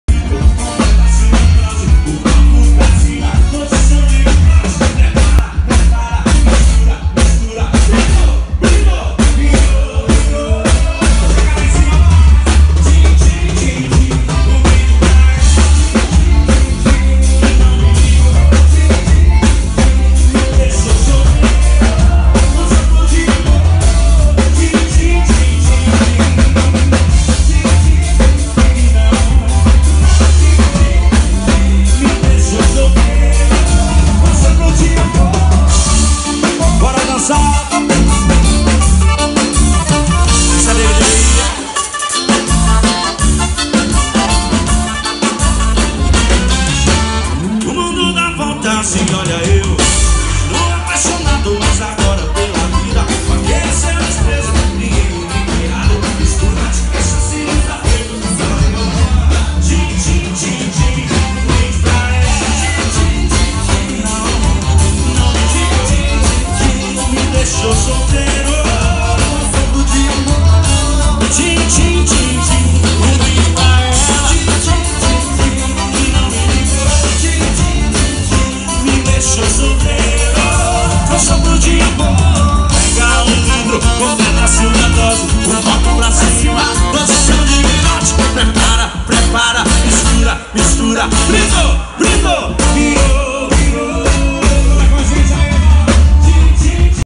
SOUNDCRAFT UI24R
Mesa RACK modelo UI24R
24 inputs
08 outputs
Gravação LR no pendrive
Gravação multi-pista